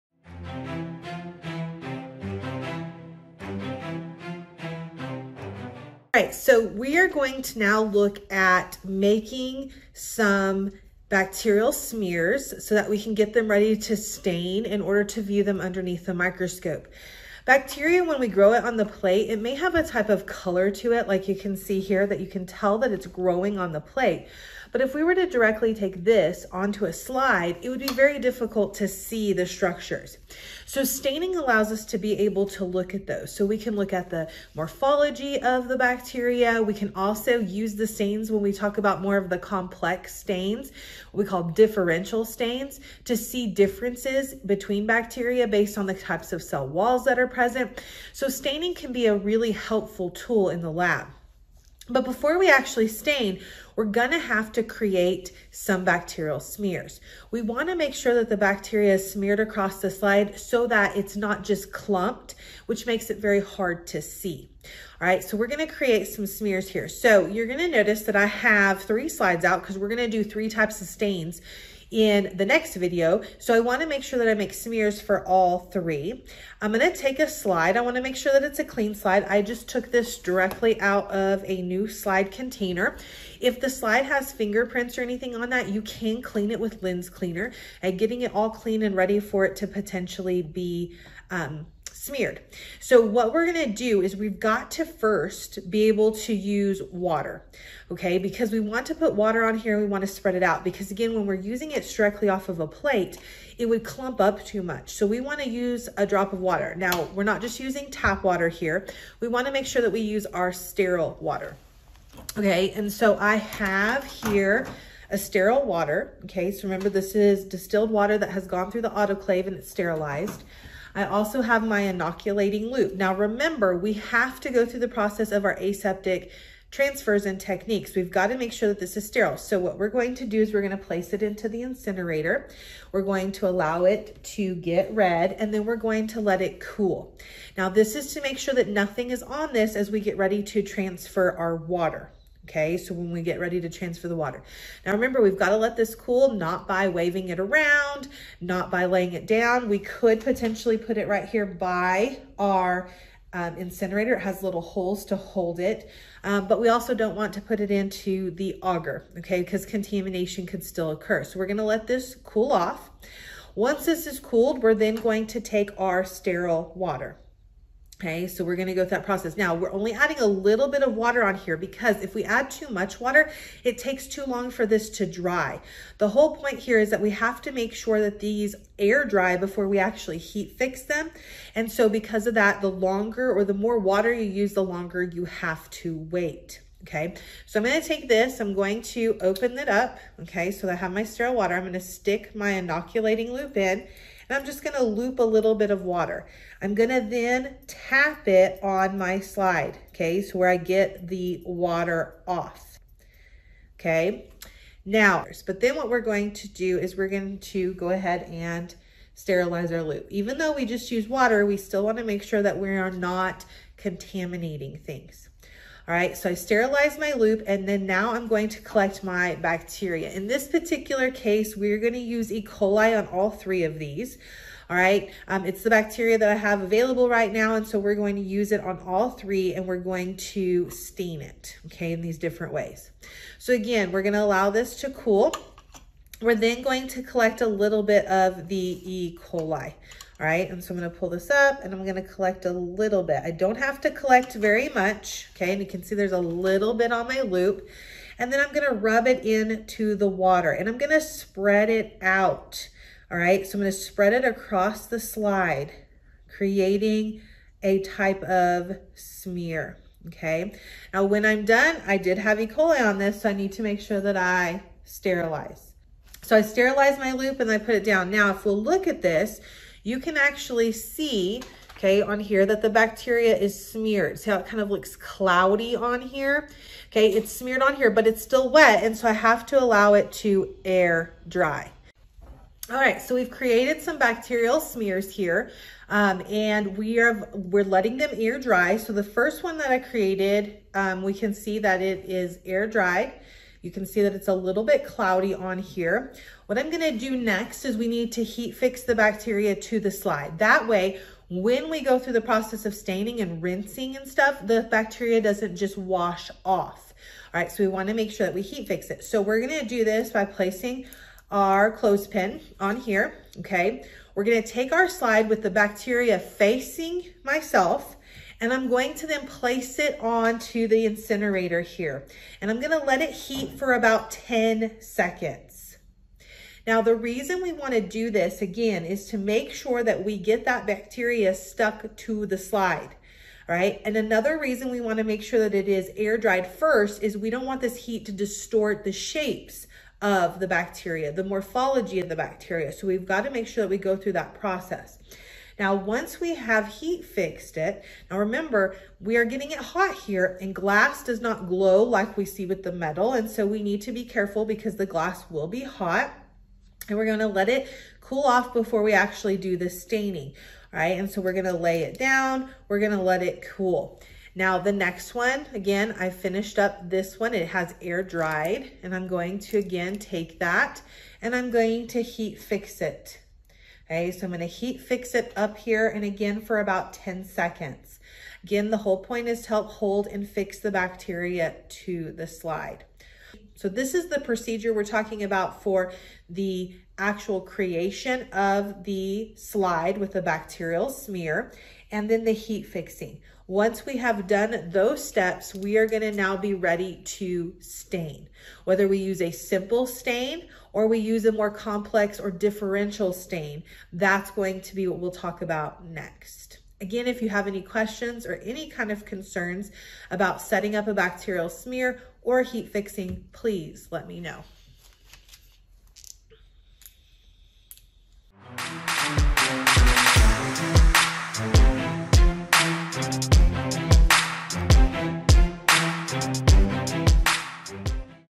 Bacterial Smear and Heat Fix
Step by step instructions on how to make a bacterial smear with heat fixing in order to prepare the slide for staining.